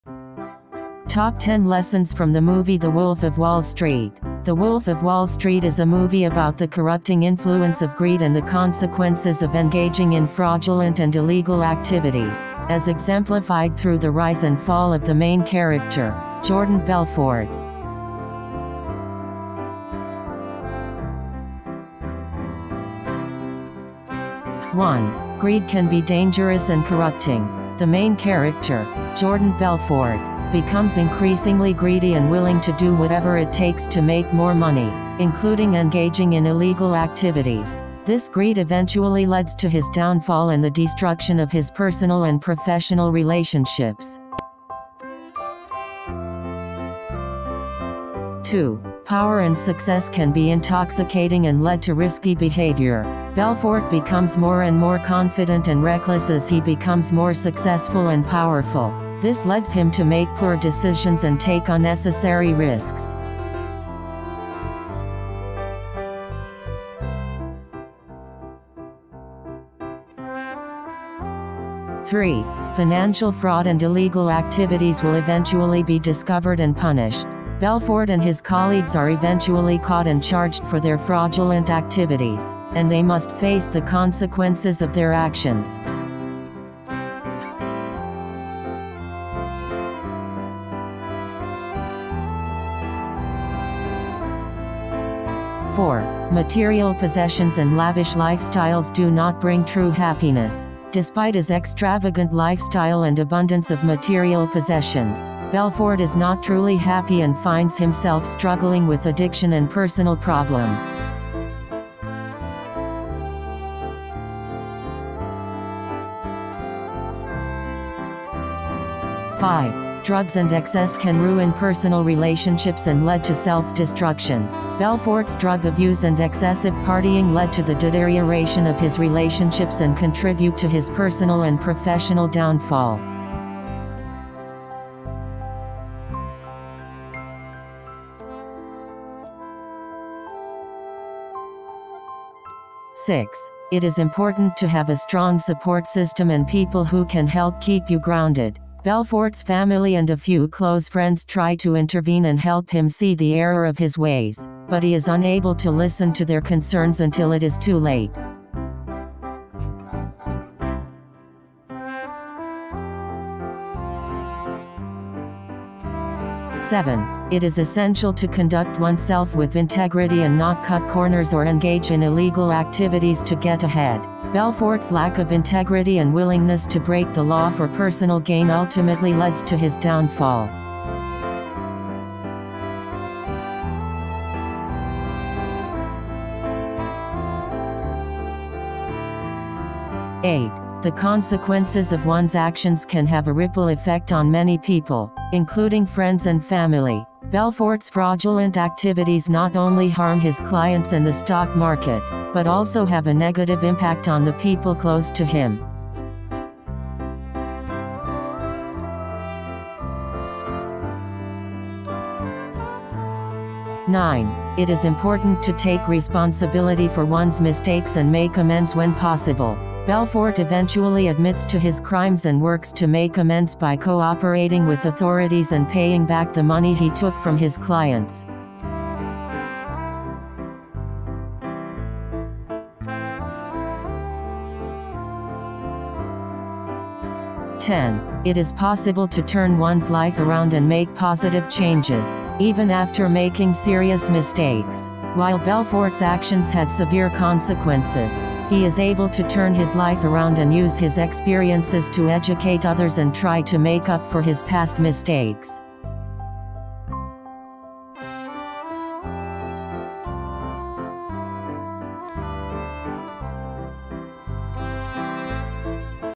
Top 10 Lessons from the movie "The Wolf of Wall Street".
Top 10 Lessons from the movie "The Wolf of Wall Street". The Wolf of Wall Street is a movie about the corrupting influence of greed and the consequences of engaging in fraudulent and illegal activities, as exemplified through the rise and fall of the main character, Jordan Belfort.
1) Greed can be dangerous and corrupting. The main character, Jordan Belfort, becomes increasingly greedy and willing to do whatever it takes to make more money, including engaging in illegal activities. This greed eventually leads to his downfall and the destruction of his personal and professional relationships.
2) Power and success can be intoxicating and lead to risky behavior. Belfort becomes more and more confident and reckless as he becomes more successful and powerful. This leads him to make poor decisions and take unnecessary risks.
3) Financial fraud and illegal activities will eventually be discovered and punished. Belfort and his colleagues are eventually caught and charged for their fraudulent activities, and they must face the consequences of their actions.
4) Material possessions and lavish lifestyles do not bring true happiness. Despite his extravagant lifestyle and abundance of material possessions, Belfort is not truly happy and finds himself struggling with addiction and personal problems.
5) Drugs and excess can ruin personal relationships and lead to self-destruction. Belfort's drug abuse and excessive partying lead to the deterioration of his relationships and contribute to his personal and professional downfall.
6) It is important to have a strong support system and people who can help keep you grounded. Belfort's family and a few close friends try to intervene and help him see the error of his ways, but he is unable to listen to their concerns until it is too late.
7) It is essential to conduct oneself with integrity and not cut corners or engage in illegal activities to get ahead. Belfort's lack of integrity and willingness to break the law for personal gain ultimately leads to his downfall.
8) The consequences of one's actions can have a ripple effect on many people, including friends and family. Belfort's fraudulent activities not only harm his clients and the stock market, but also have a negative impact on the people close to him.
9) It is important to take responsibility for one's mistakes and make amends when possible. Belfort eventually admits to his crimes and works to make amends by cooperating with authorities and paying back the money he took from his clients.
10) It is possible to turn one's life around and make positive changes, even after making serious mistakes. While Belfort's actions had severe consequences, he is able to turn his life around and use his experiences to educate others and try to make up for his past mistakes.